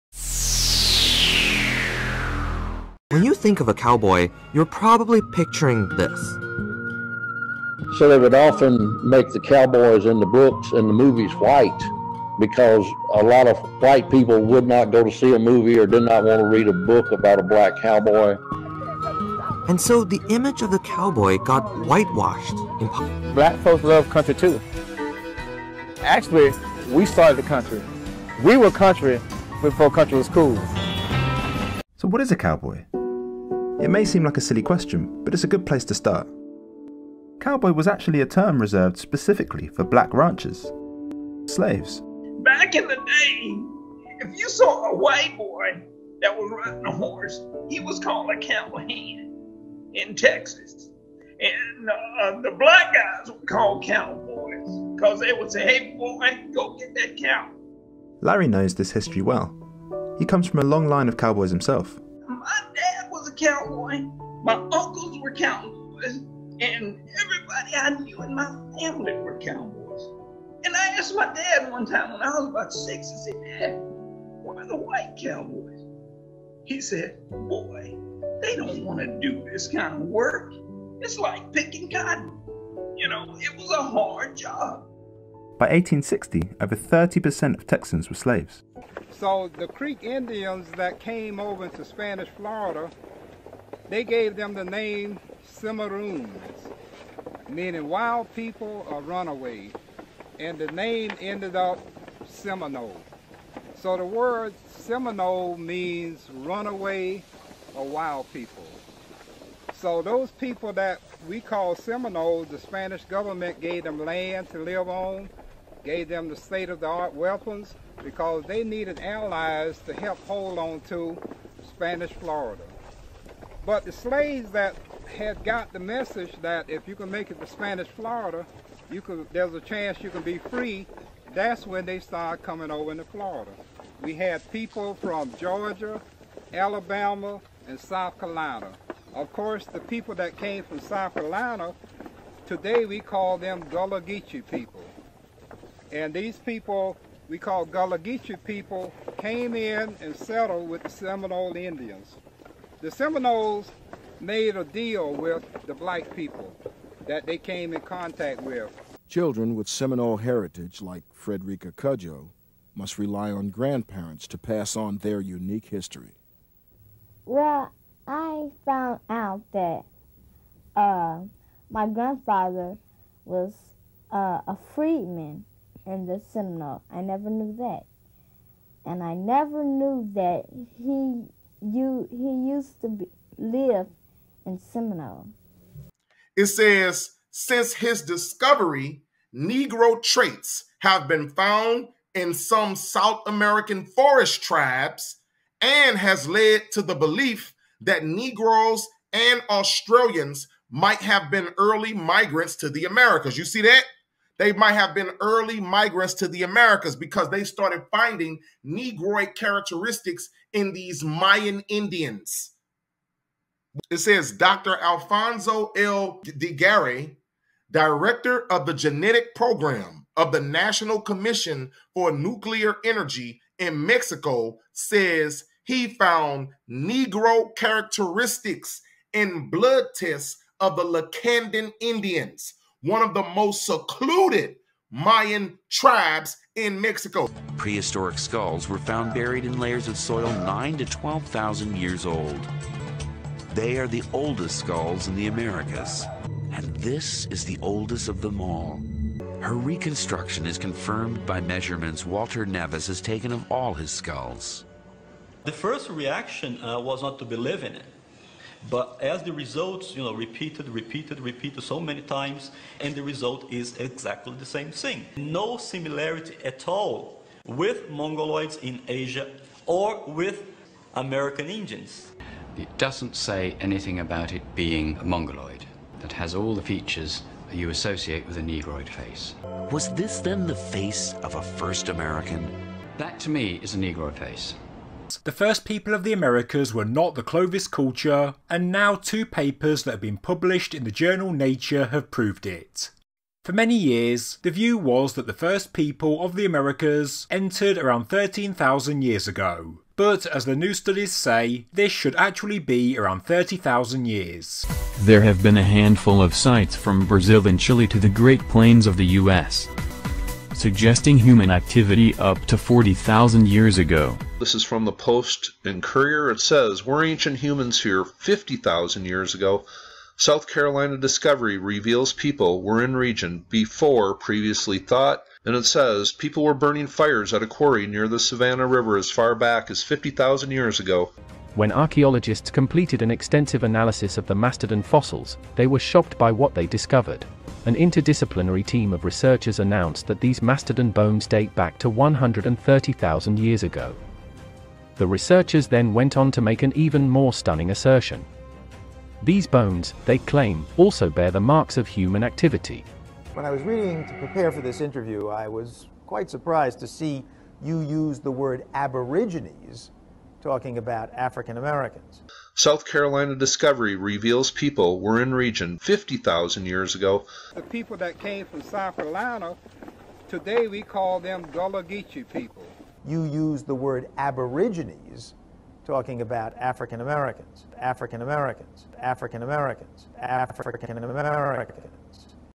The First Cowboys and Indians were Negro so called FBA
-- mirror --
-- NORTH AMERICAN NEGRO NATION --
-- still more evidence of not only so called blk artifacts over here, but so called blk civilizations some over a couple thousand years old in america.. gettin interesting and makes you wonder why they are supper hiding this stuff.
ap TMH Almighty